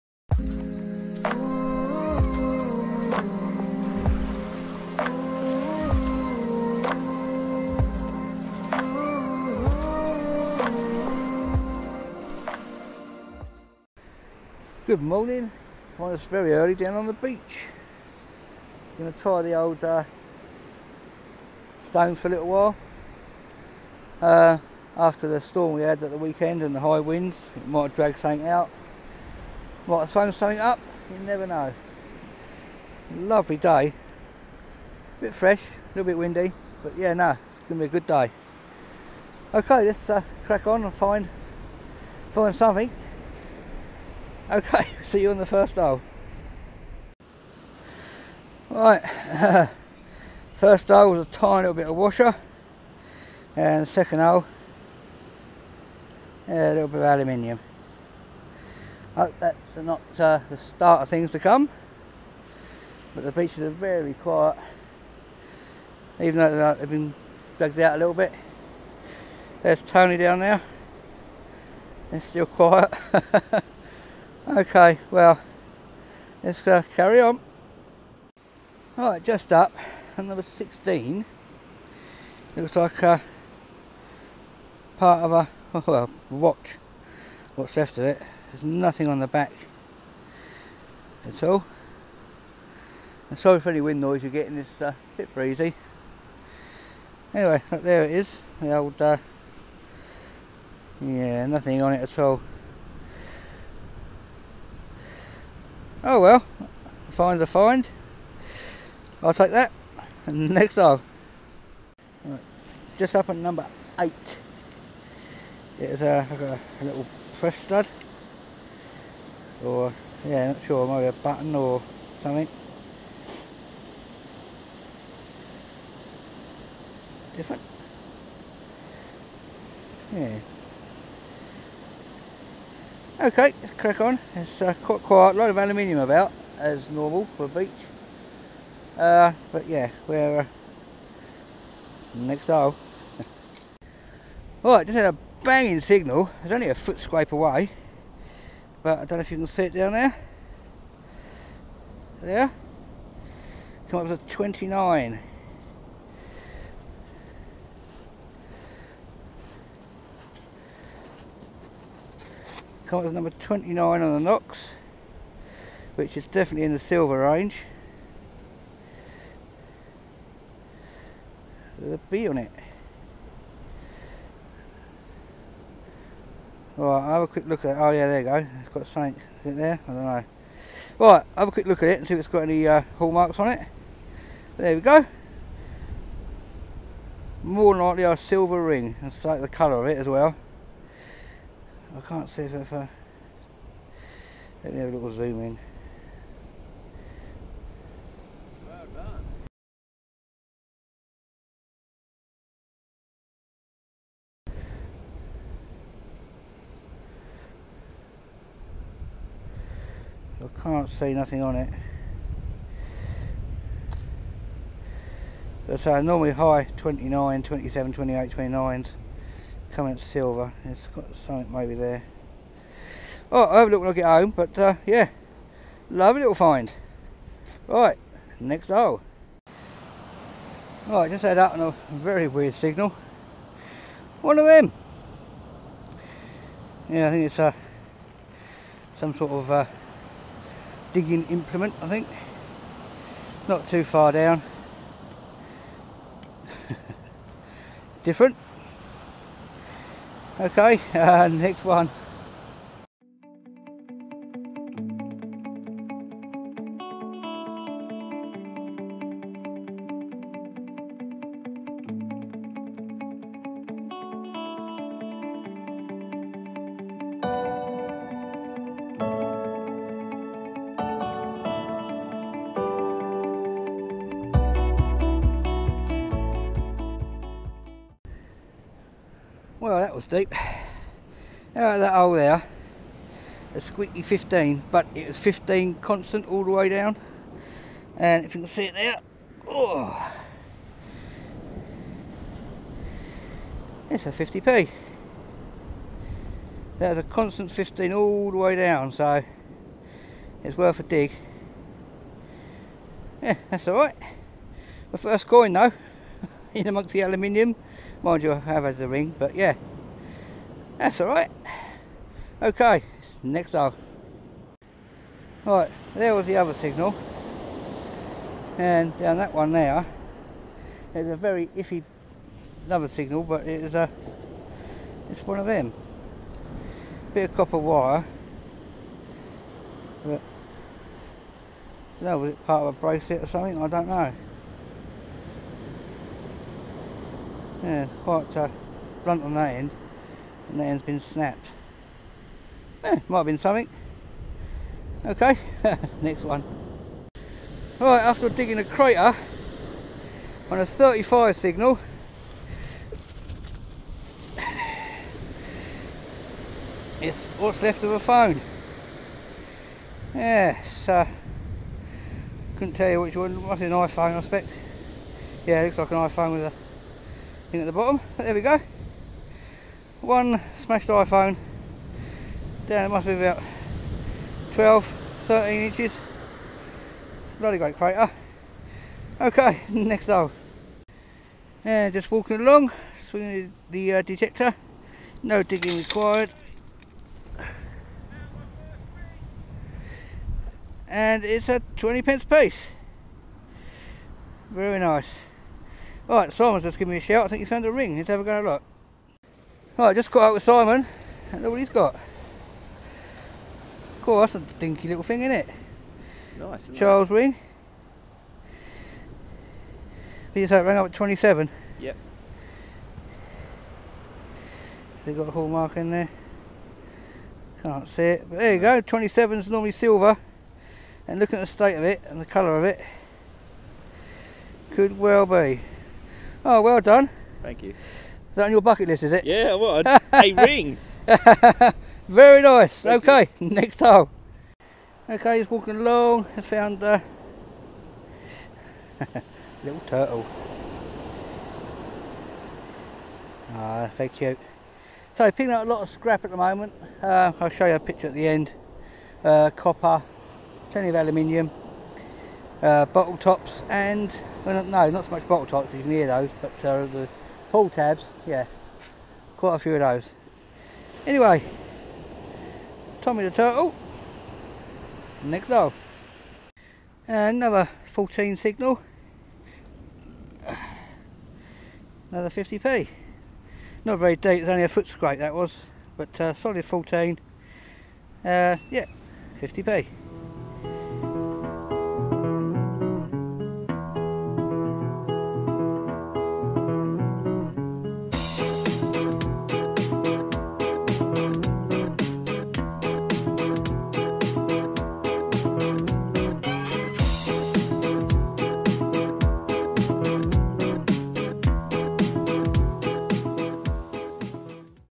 Equinox 800 back on the beach - finds Silver :) | 119
Had a great morning down a local beach with Tony and Simon with the Nox 800
It was hard going but Simon and myself managed to find Silver :)
We were all using the Equinox 800.
If you need a great machine for the beach & land then the Equinox 800 or Nox 600 is highly recommended.

ATB & HH

Lee